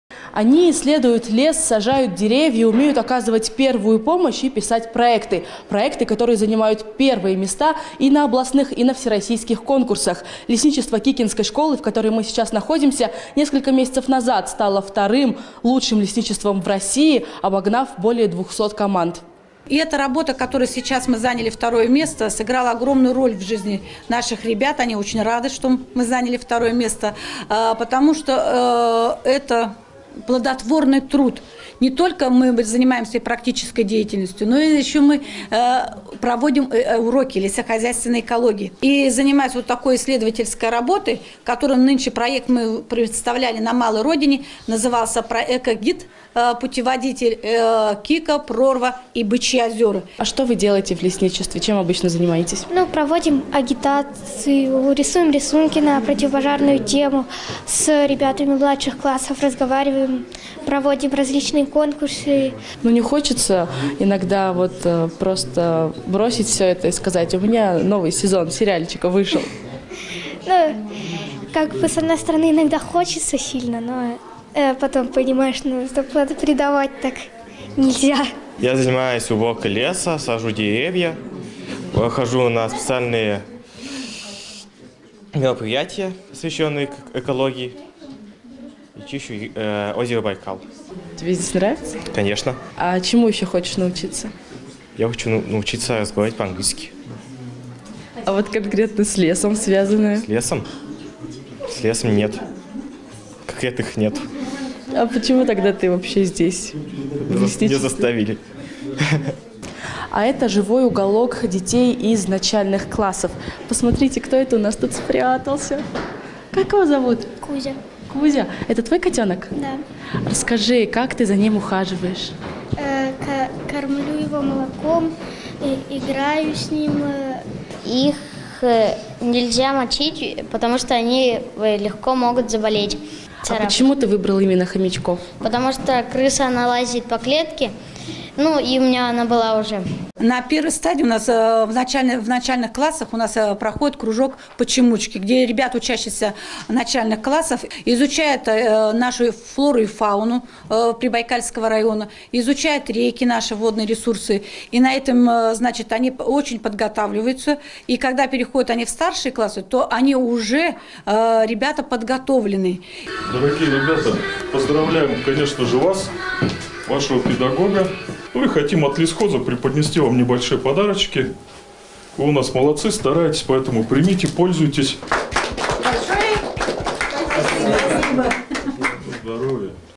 Школьное лесничество из Бурятии стало одним из лучших на всероссийском конкурсе
Юные лесники из Бурятии взяли серебро на всероссийском конкурсе лучших школьных лесничеств. Участники из села Кика Прибайкальского района представили свой проект и получили заслуженную награду. Это одно из немногих лесничеств, которое существует свыше 40 лет. За эти годы много поколений детей проделали огромную работу.